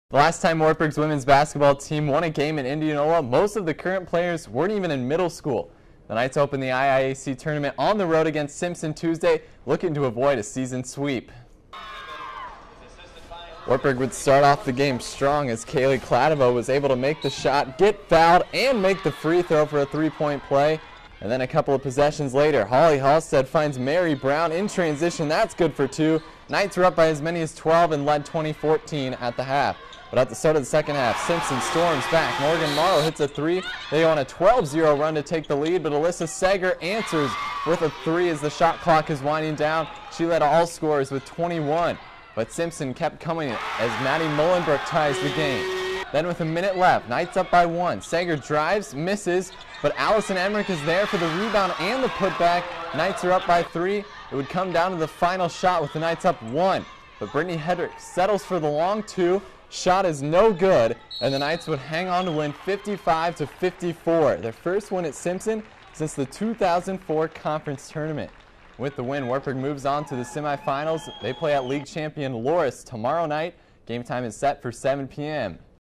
Wartburg Women's Basketball at Simpson IIAC Quarterfinals (Feb. 26, 2014)
Wartburg advanced to the semifinal of the Iowa Conference tournament with its 55-54 win over Simpson.